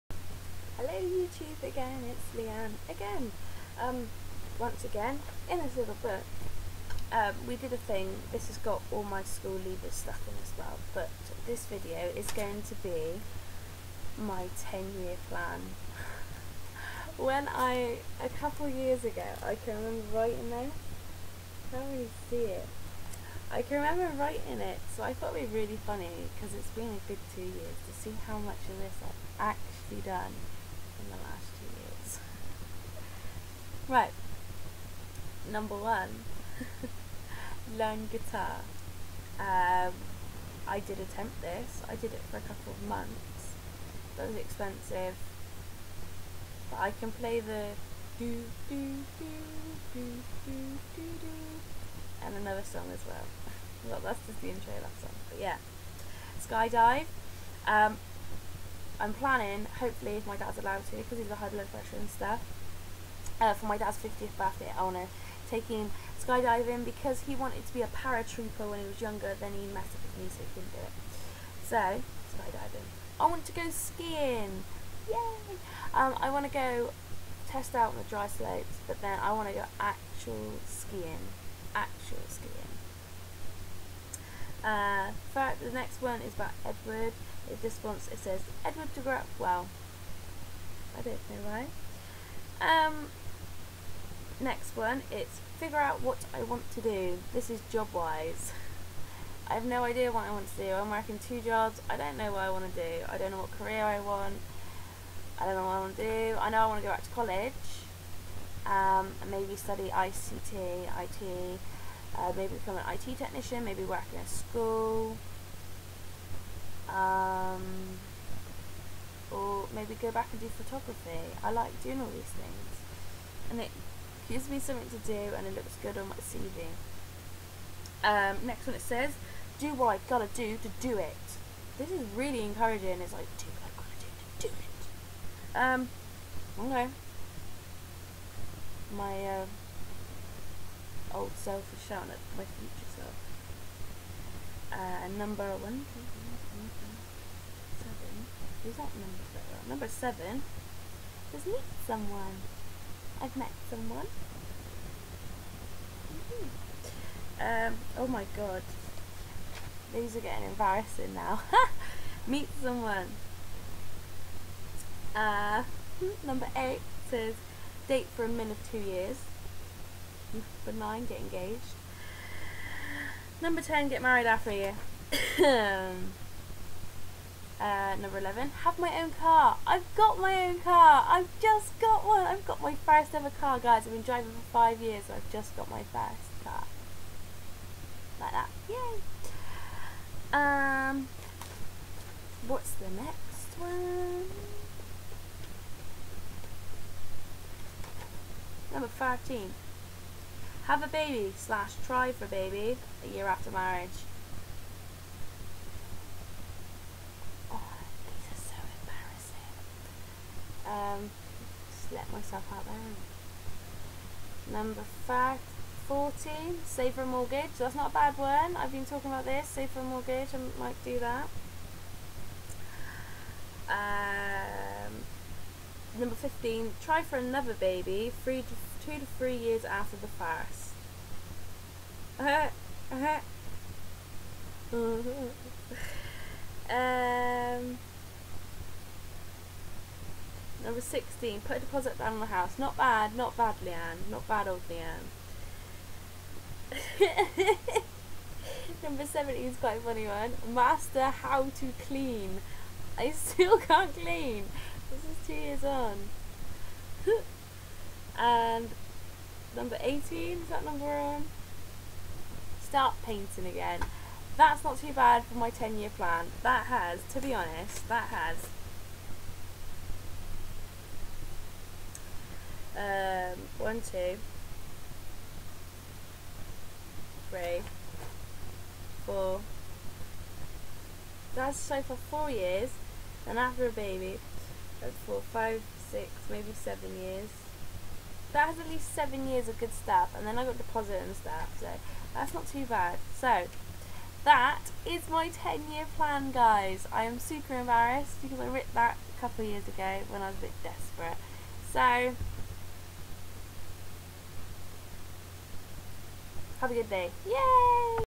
Ten Year Plan
after finding this book and found my ten year plan i did when eddie was small

i dicided to see how much of it i have done or what i dont wonna do any more